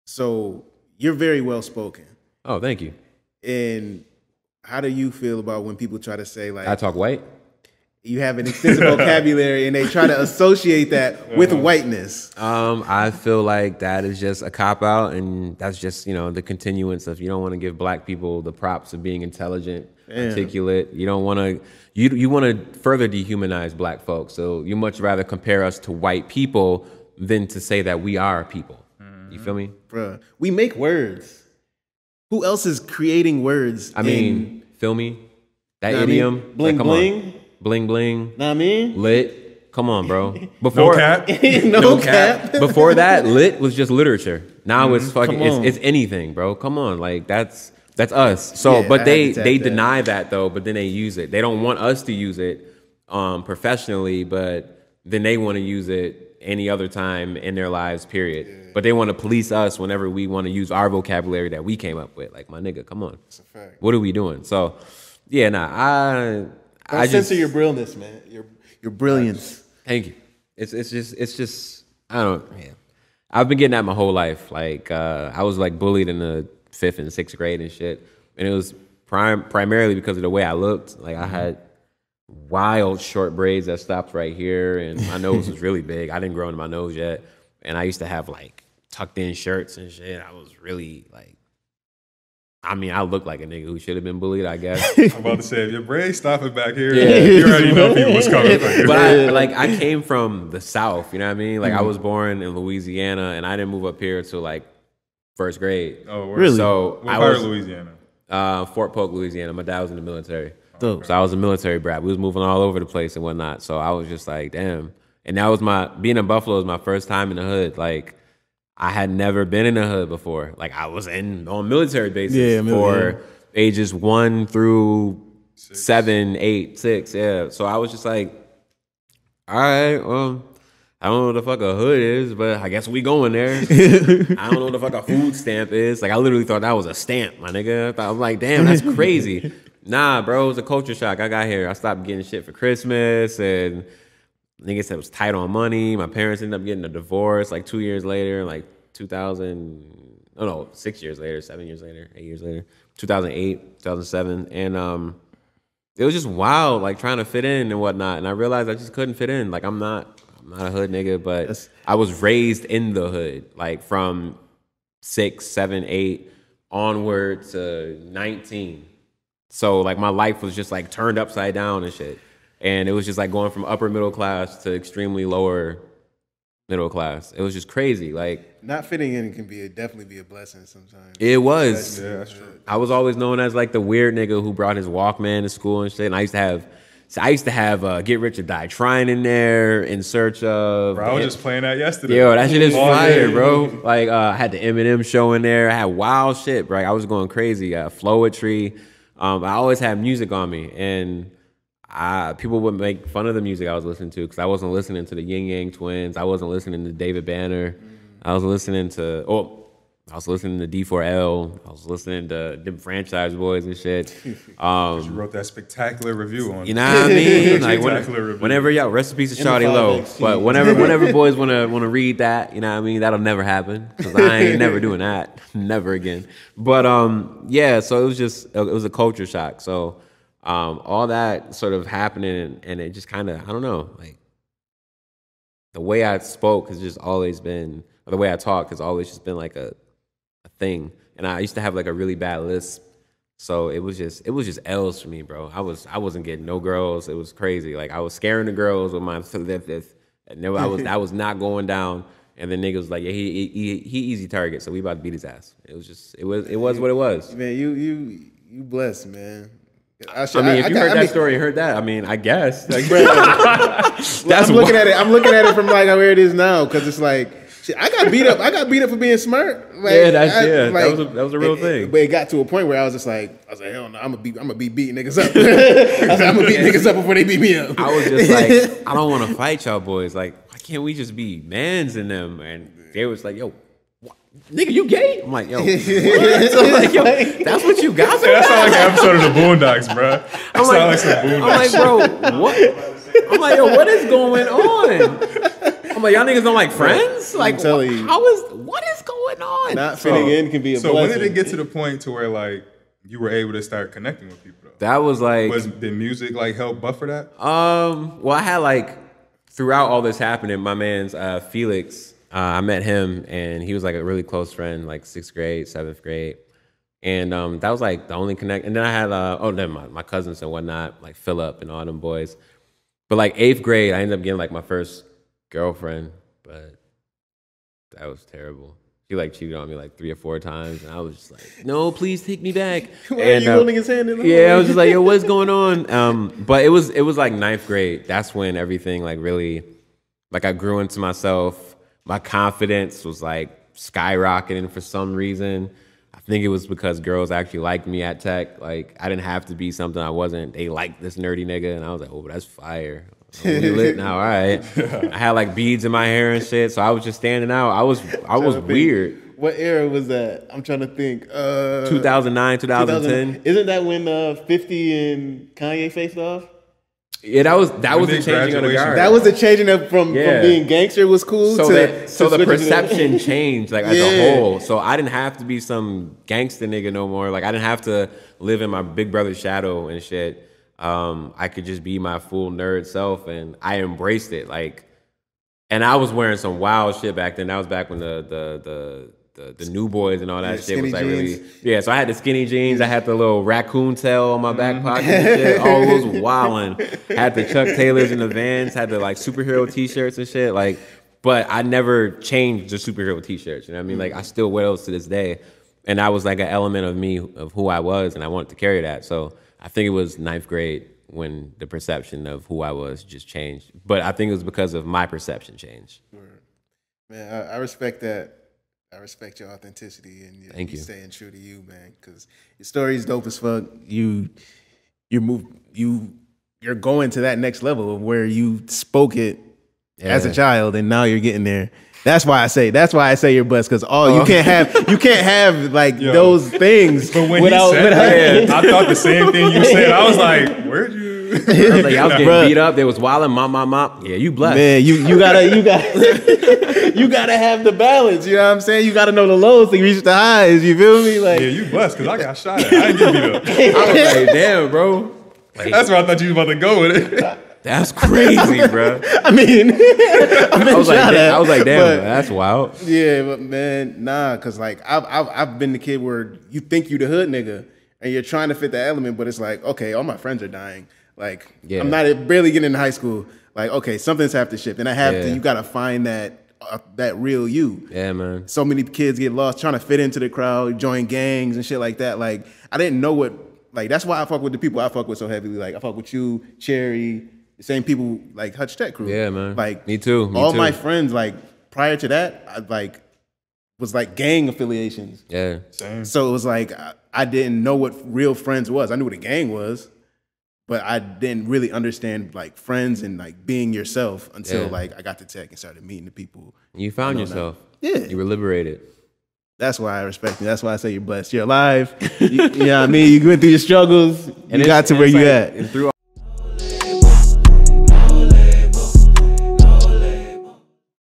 IMPORTANCE OF FINDING YOURSELF | MARC GRIFFIN
Clip from SZN 4 EP 13 w/ MARC GRIFFIN.  MARC shares some of his trials and tribulations and the importance of finding yourself.